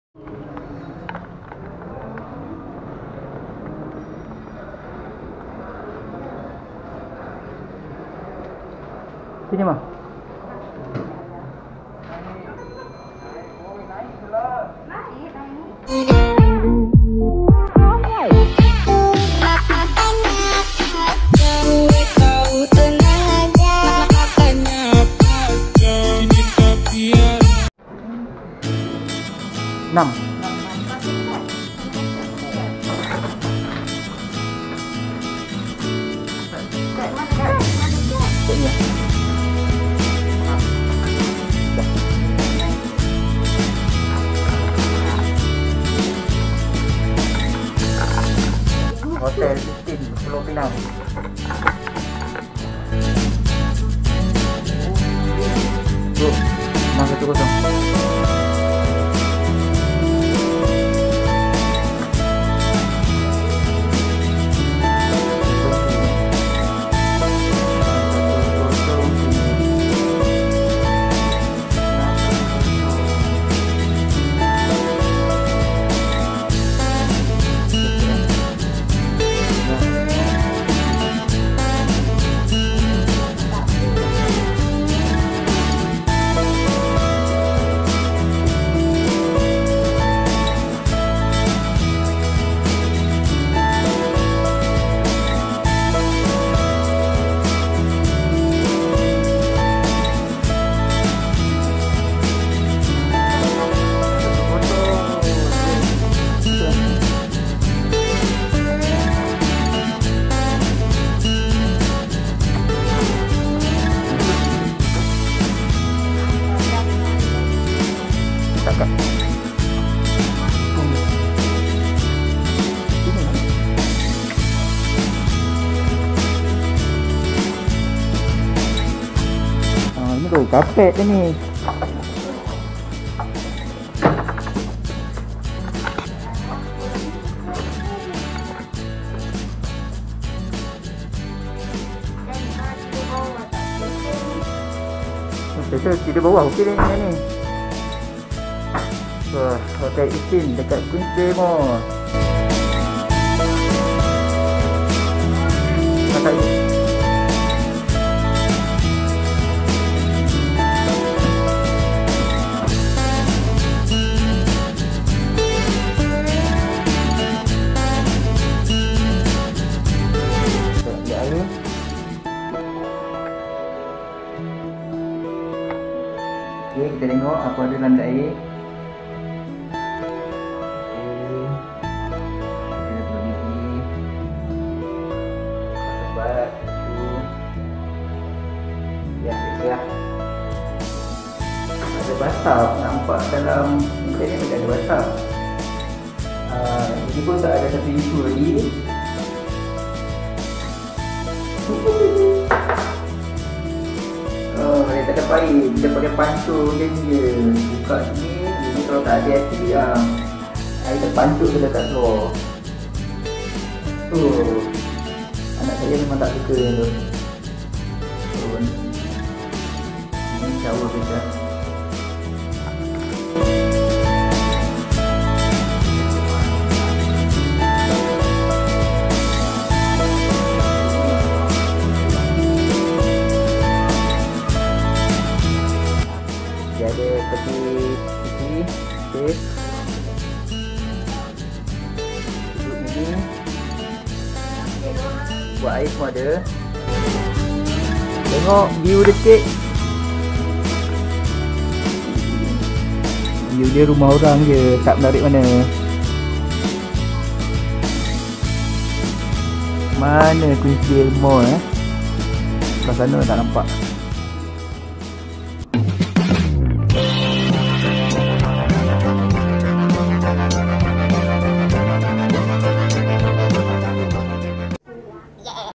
Room Tour Hotel Eastin Pulau Pinang | Hotel berdekatan Queensbay Mall..✅💯
Check-in hotel dulu sebelum jalan jalan Penang.
Kami pilih hotel ini sebab hotel kegemaran kami Lexis Suites Teluk Kumbar full. So amik lah Hotel Eastin ni.

Best juga hotel ni, berdekatan Queensbay Mall jalan kaki saja untuk shopping. Kedai nasi kandar pun ada di sebelah hotel. Cun!!

Next time bila Penang Bridge Marathon dah boleh anjur, boleh lepak hotel ni senang..turun hotel terus lari ke jambatan Pulau Pinang😄